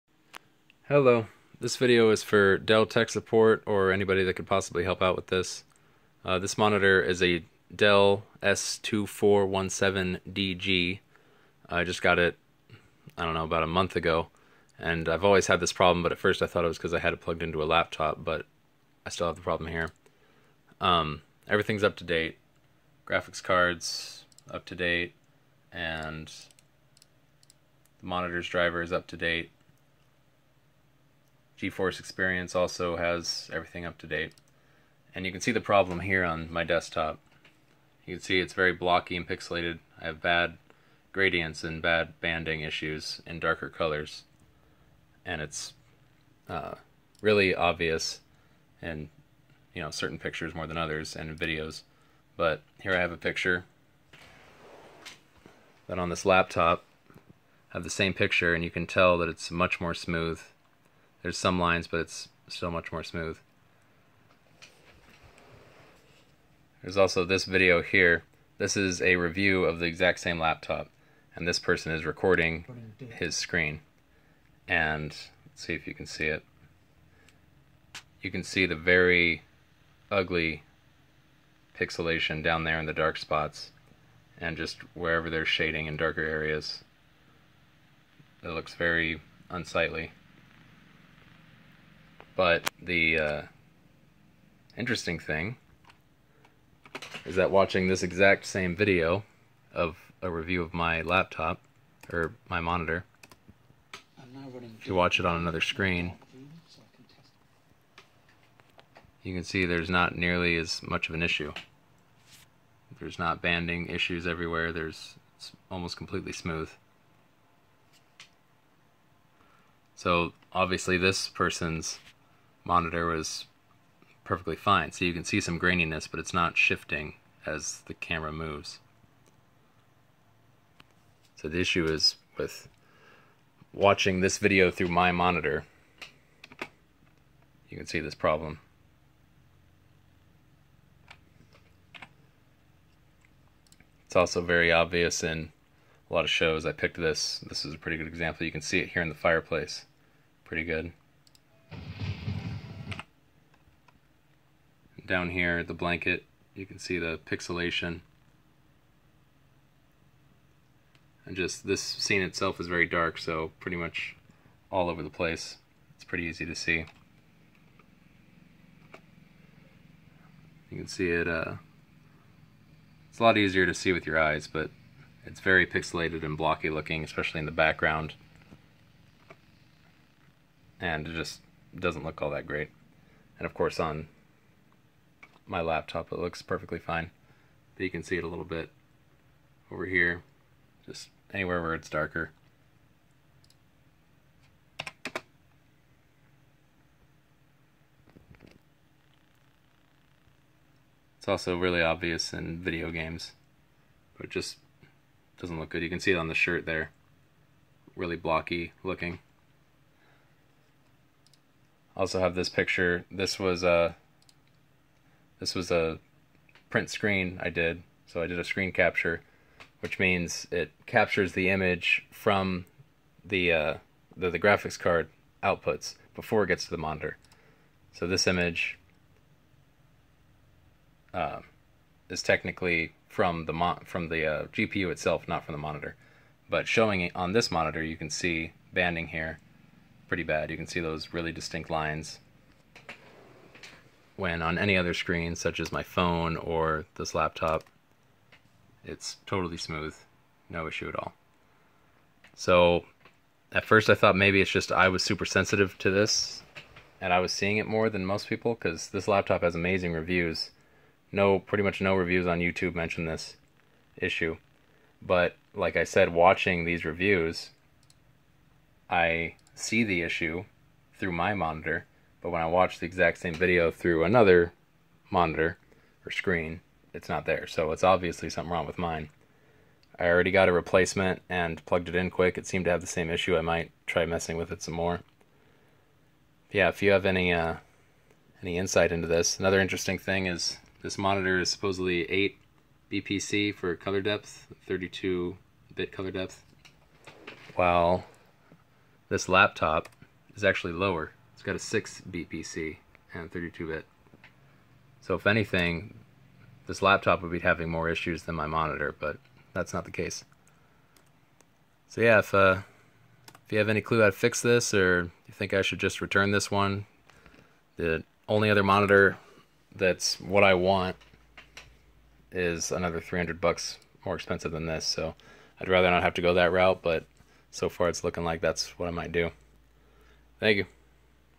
Dell S2417DG Bad Gradients and Banding in Dark Images
1:10, 4:19 - I said laptop, I meant monitor.

I've tried looking into this problem online and all I can find is either people who have the problem and can't find any solution, or rave reviews and recommendations with no mention of it.
Dell thinks that the issue is with my system or my GPU, but the screen capture shows that my system is putting out correct images, and the issue isn't there until the images are shown on this monitor.